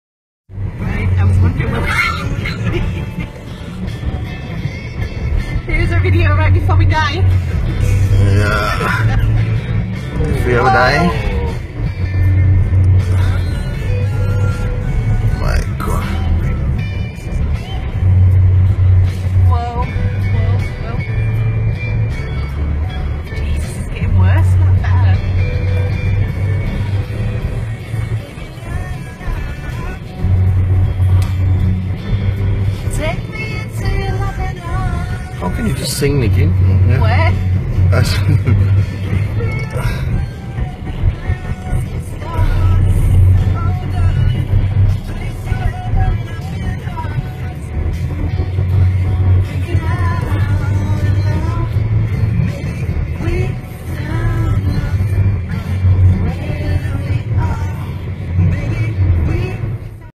Iceland Karaoke Snow Pool
We like to call this one Karaoke Snow Pool. Turn up your volume if you want to hear the dialog in the car (or the singing. ). This was taken driving back from the Golden Circle to Reykjavík.
The quality is not that great (sorry) but you will get the gist. And, no stopping in a storm like this is not an option.